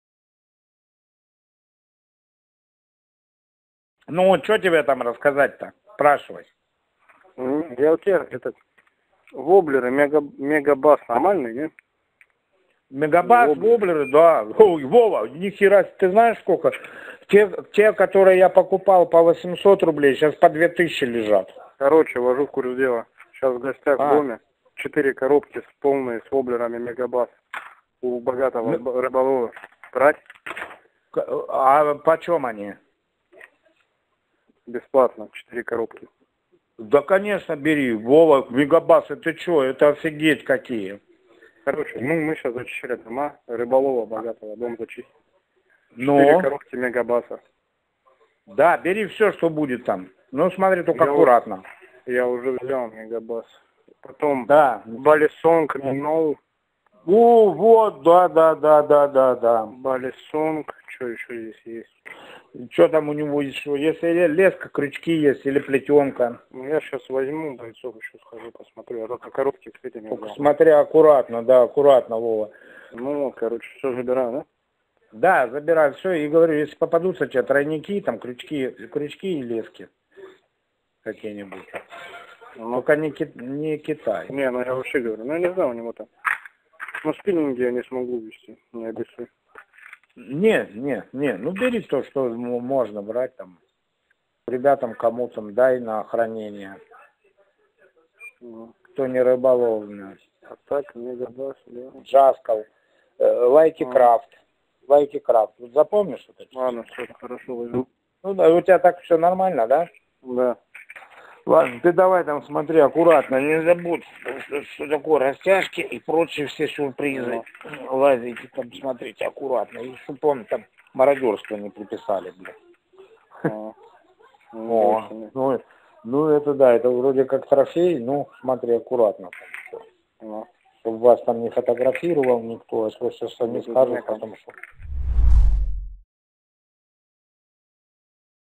Родичі окупантів чекають їх не з перемогою, а з награбованими речами
Мародерство, до речі, називають «спецоперацією із зачистки домов». Про це йдеться у нових телефонних розмовах загарбників, які є у розпорядженні СБУ.

На записі чути, що деякі рашисти приймають замовлення від близьких, як перед походом до супермаркету.

Як ось цей «герой»: зателефонував своєму батьку прямо з будинку, який грабував.

Порадував батька-рибалку, що потрапив до оселі «багатого рыболова». Останній, як почув перелік фірм виробників рибальського спорядження, аж розхвилювався: «…ни х*ера се! Ты знаешь, сколько те воблеры, которые я покупал по 800 рублей? Сейчас по 2 тысячи лежат».

Дізнавшись, що там такого добра чимало, наставляє сина: «Да, бери все, что будет там, но смотри только аккуратно….чтоб мародерство вам там не приписали, бл*дь. …это, вроде как трофей, но чтоб вас там не фотографировал никто».

Що ж, наші ЗСУ вас, окупанти, точно не фотографуватимуть. А от в бою будуть нищити – за те, що ви робите на нашій землі!

Відео: СБУ